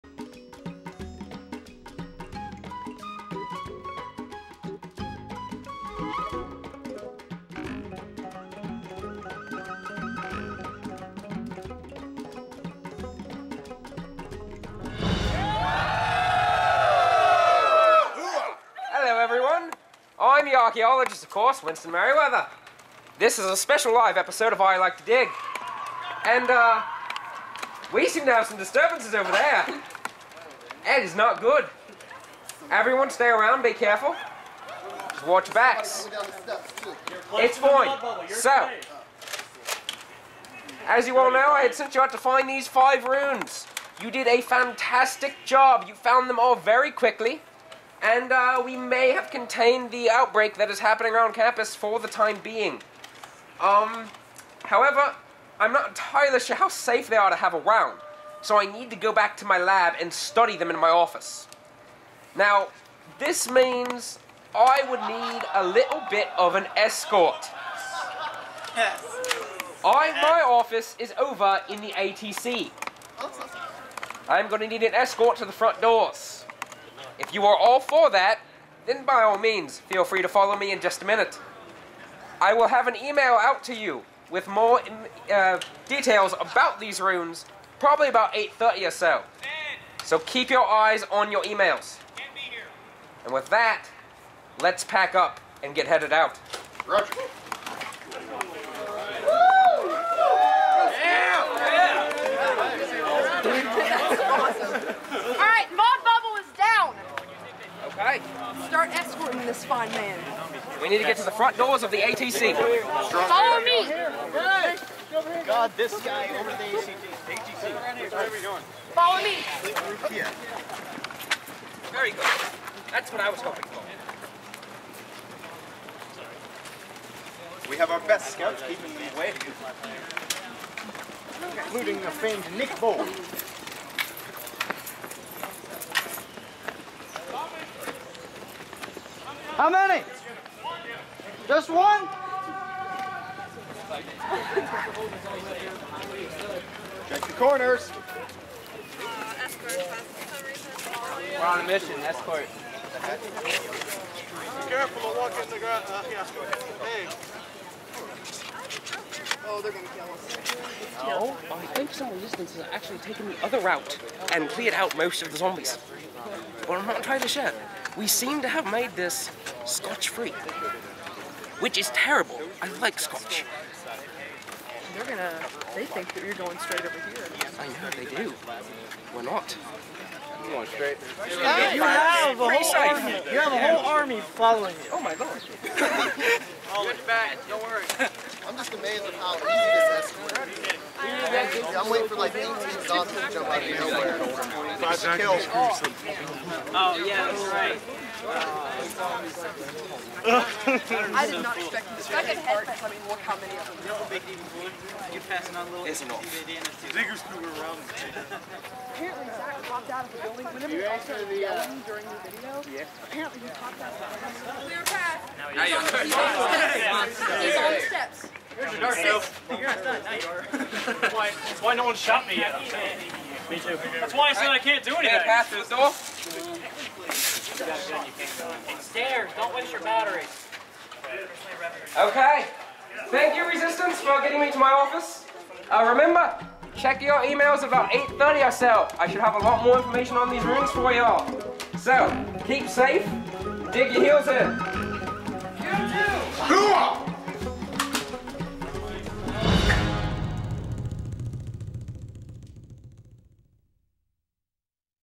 SSU Hvz Fall 2012 - Escort after Mission 1
This was at the end of Mission 1 and was a slight setup for Mission 2